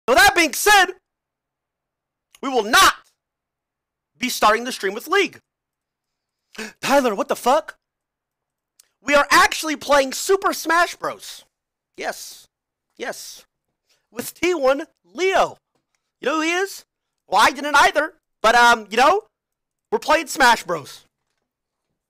Twitch Ban new "S" word against Tyler1 as a derogatory Slur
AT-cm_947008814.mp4